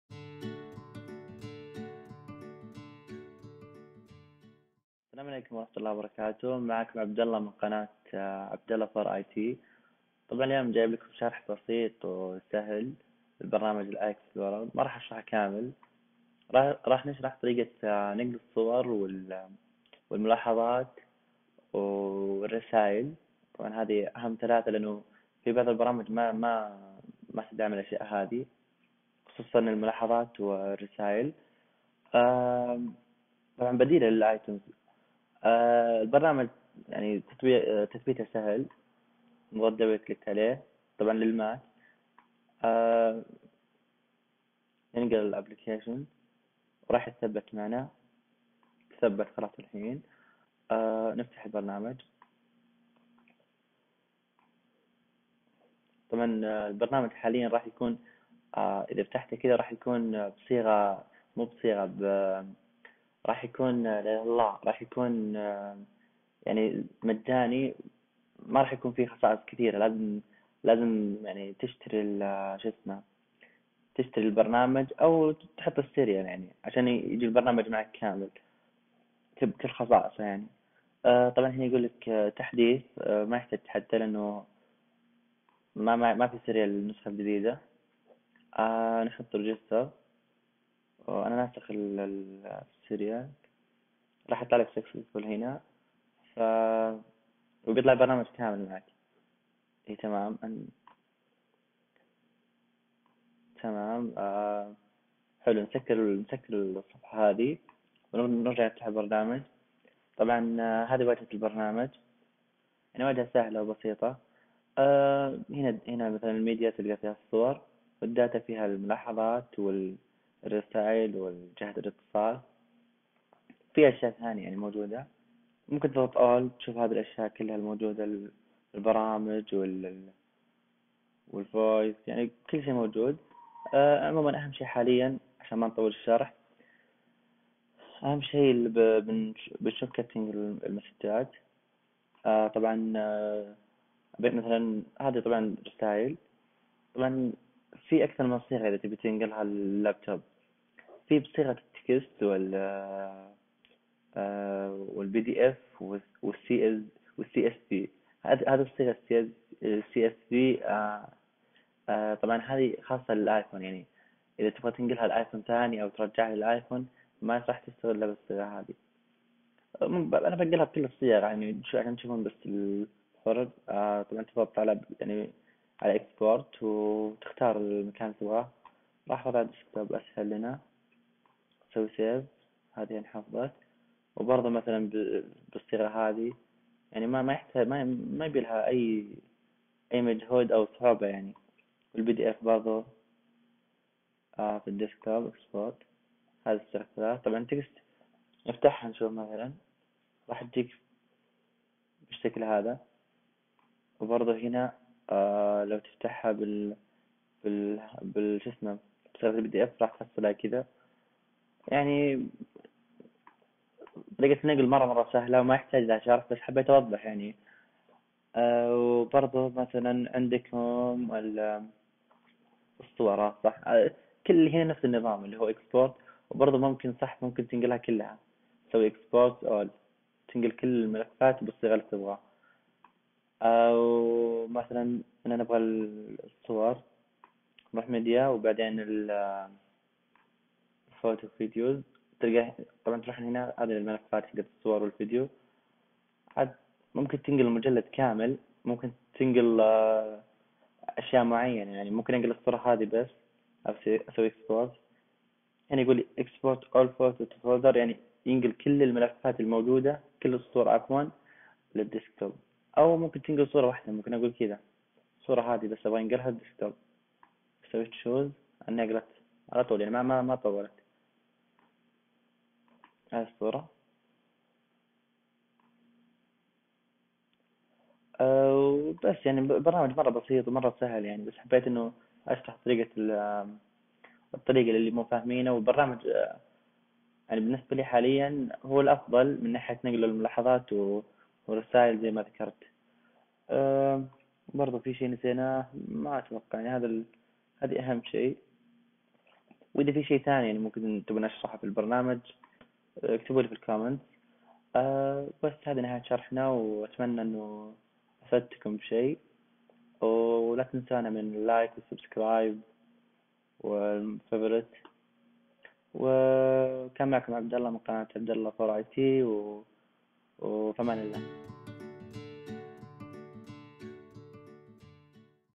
شرح برنامج iExplorer لنقل الملاحظات والرسائل والصور من الايفون للاب توب - للماك
ـــــــــــــــــــــــــــــــــــــــــــــــــــــــــــــــــــــــــــــــــــــــــــــــــــــــ

شرح بسيط لنقل الملاحظات والرسائل والصور من الايفون الى اللاب توب بكل سهوله
عن طريق برنامج iExplorer

* اول شرح لي ، اعتذر عن الاخطاء واتمنى انكم استفدتو ولو شيء بسيط

دعواتكم ونشوفكم في شرح ثاني بإذن الله =)

ــــــــــــــــــــــــــــــــــــــــــــــــــــــــــــــــــــــ